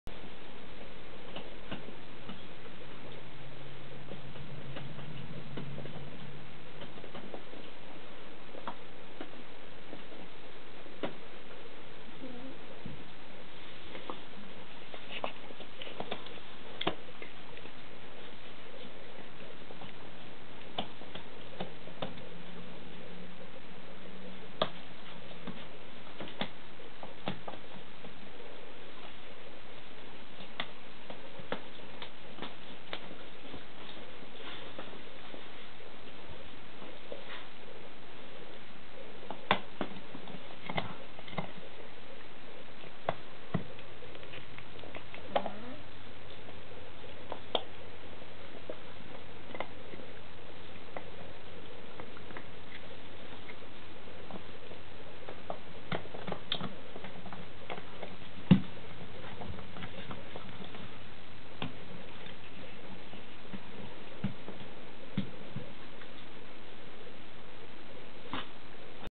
Standard Schnauzer Flute hanging with pups.mov
6 week old standard schnauzer puppies sleep while their Auntie Flute enjoys chewing on a cow's hoof.